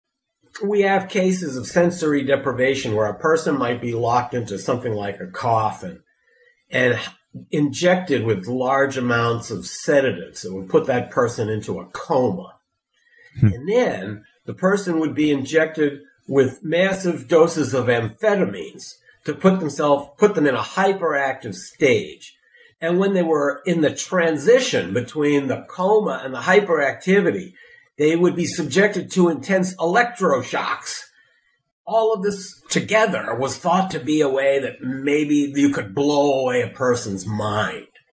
An example of torture in the CIA's "MK-Ultra" program | Stephen Kinzer
Stephen Kinzer is a former "New York Times" correspondent, a Senior Fellow in International and Public Affairs at Brown University, and is the author of "Poisoner in Chief: Sidney Gottlieb and the CIA Search for Mind Control."